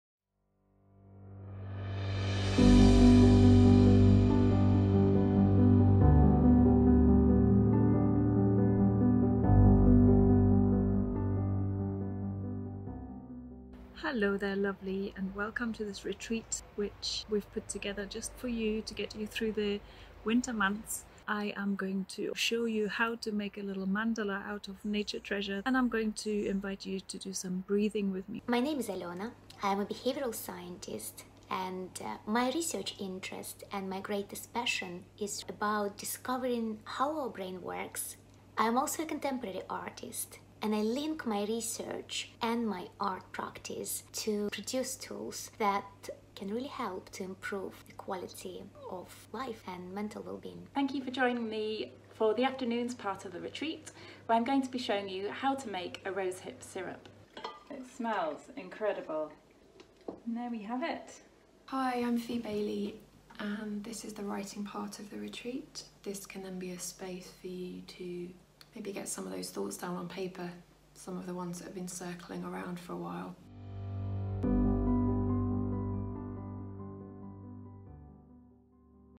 Winter Connections Trailer
Take a snapshot view at our Winter Connections online wellbeing retreat that we prepared for you to feel mentally better this winter. You can see more details and buy this 1-day programme